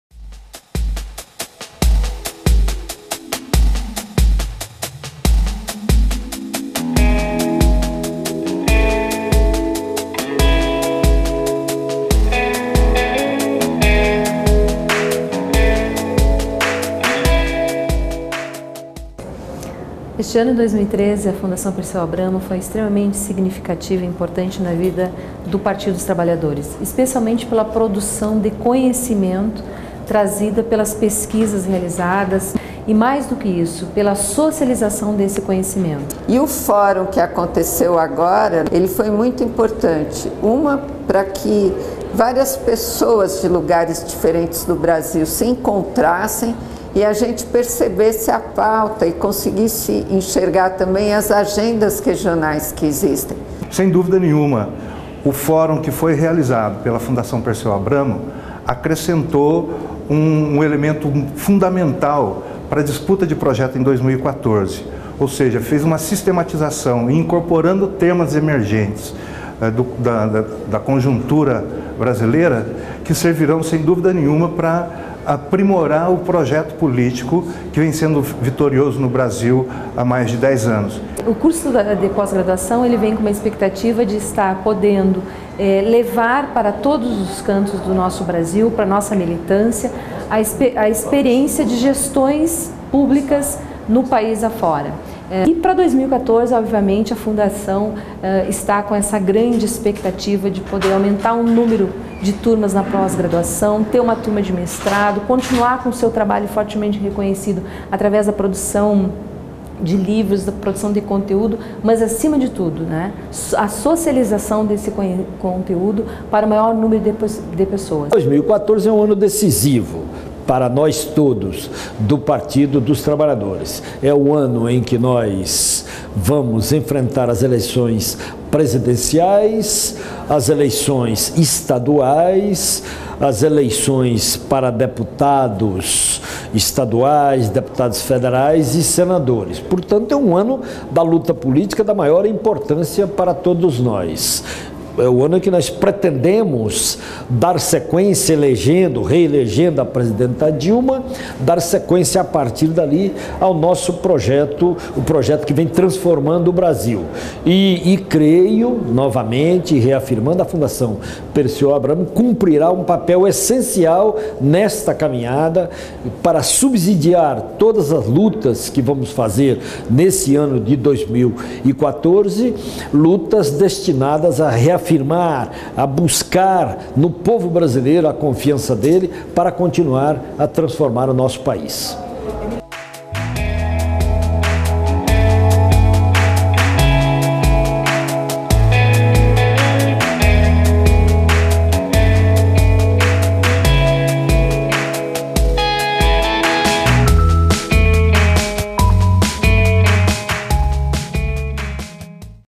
Reunião do conselho curador da FPA realizado em Brasília.
O conselho curador da Fundação Perseu Abramo, em reunião realizada em Brasília, fala sobre a importância do trabalho realizado em 2013 e os novos rumos para 2014.